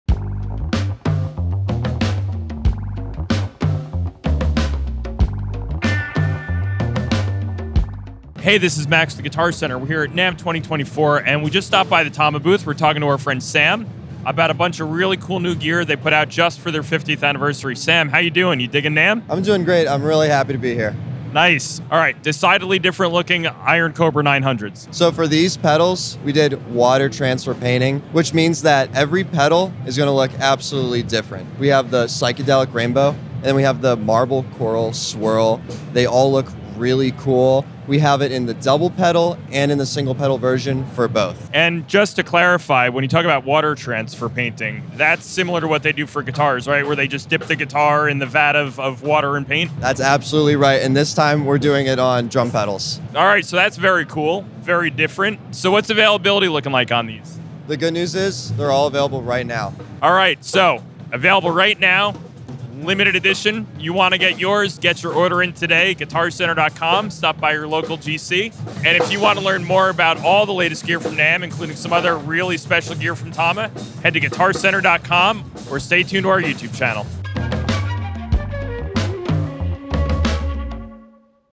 TAMA Limited-Edition 50th Anniversary Iron Cobra Power Glide Pedal | New from NAMM 2024
Dig in now with the Iron Cobra Power Glide from Guitar Center

The Power Glide also comes in a double-pedal version.

The Iron Cobra series of kick pedals has been a top choice of professional drummers for many years. Users include percussion heavies across all styles of music, such as Lars Ulrich, Stewart Copeland, Simon Phillips, Peter Erskine, The Pocket Queen (Taylor Gordon) and many other notables.

The Iron Cobra Power Glide is renowned for its smooth-as-silk action, speed and reliability. This is a pedal you can take on tour, play a seemingly never-ending run of punishing club dates and bring to studio session after studio session with practically zero concern it will go down in the middle of a show. For a measure of fun and exclusivity, the Limited-Edition 50th Anniversary version Power Glide is decorated with a Coral Swirl finish over a black base. The quasi-psychedelic collage of color is pretty cool, but even hipper is the fact that no two finishes are alike. Your pedal is yours and yours alone.